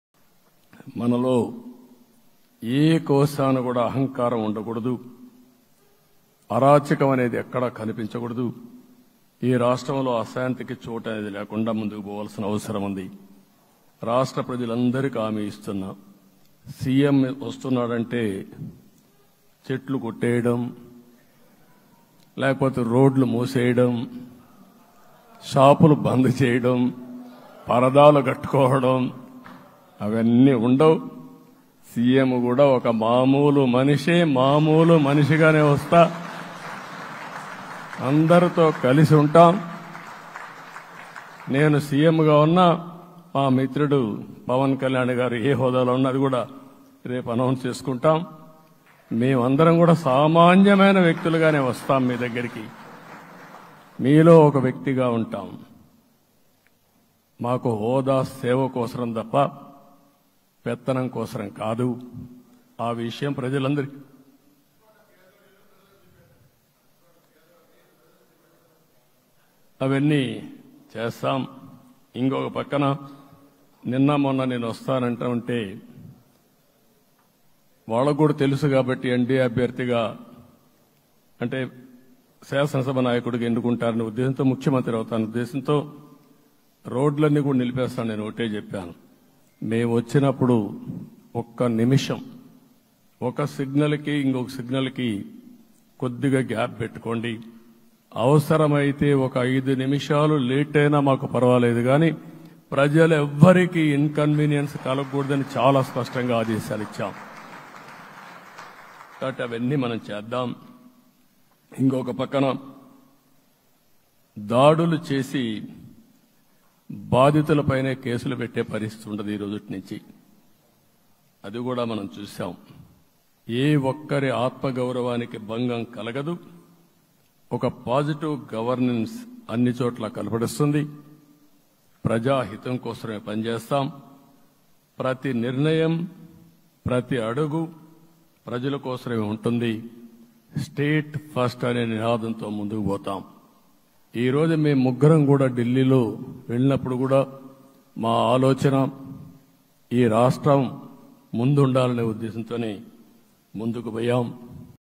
నేను మామూలు మనిషిగానే వస్తా.. అందరితో కలిసి ఉంటా
గతంలో సీఎం వస్తే చెట్లు కొట్టివేత, షాపుల బంద్ జరిగేది. గతంలో సీఎం వస్తే పరదాలు కట్టడం వంటివి జరిగేవి. నేను మామూలు మనిషిగానే వస్తా.. అందరితో కలిసి ఉంటా . మేమందరం సామాన్య వ్యక్తులుగానే మీ వద్దకు వస్తాం. హోదా అనేది సేవ కోసం తప్ప.. పెత్తనం కోసం కాదు. నేను వస్తుంటే ట్రాఫిక్ ఆపుతున్నారు. ఎక్కువ సేపు ప్రజలను ఇబ్బంది పెట్టొద్దని, సీఎం వస్తే ప్రజలకు ఇబ్బంది కలగకూడదని ఆదేశాలిచ్చా. ఏ ఒక్కరి ఆత్మగౌరవానికి భంగం కలగకూడదు.